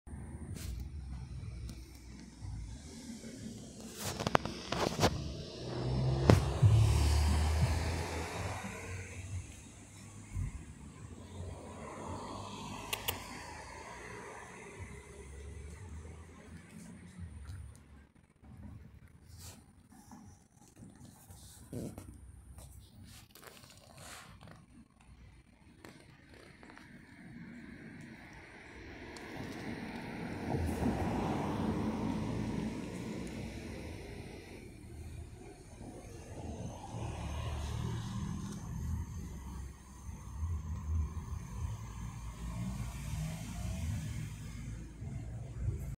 Jesse On The Pink Backpack
Jesse, like all the cats, loves my pink backpack:)

Meow!